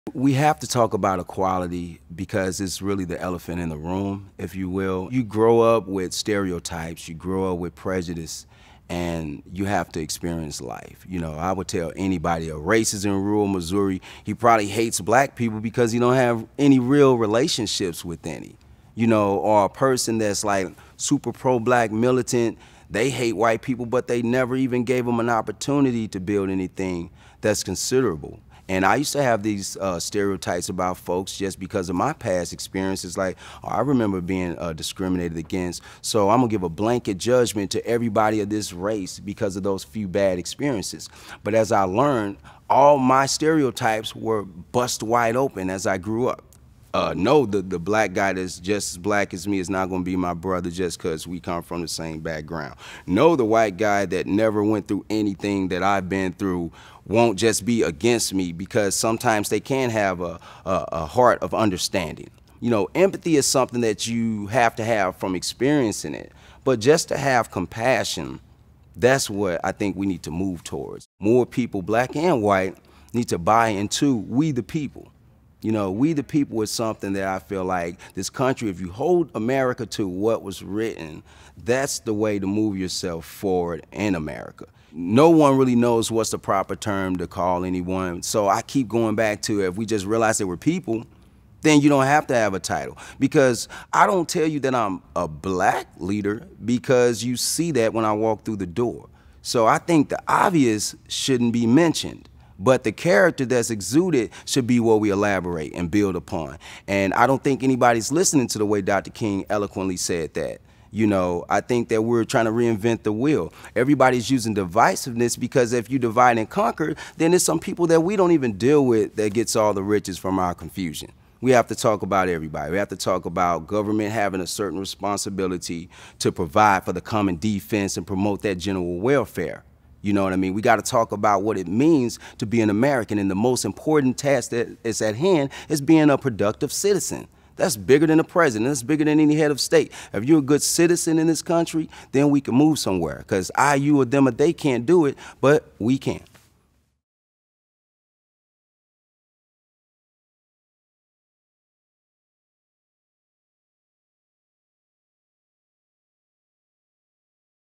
Whatz It Feel Like: Equality
Brother Dennis describes how his stereotypes changed as he got older and met other people from other backgrounds. He also explains why empathy and compassion are key when it comes to achieving equality.